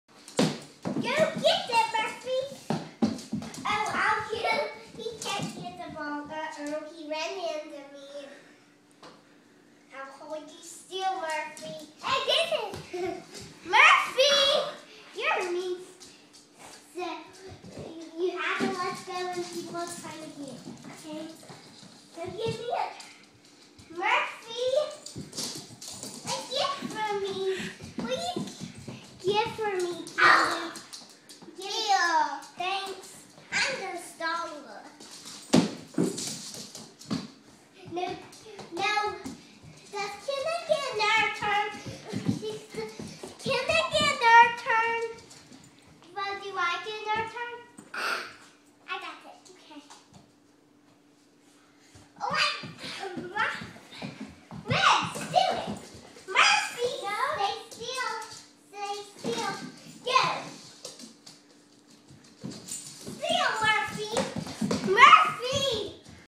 Murphy's new toy
Camden and Natalie help Murphy play with the new toy they gave him.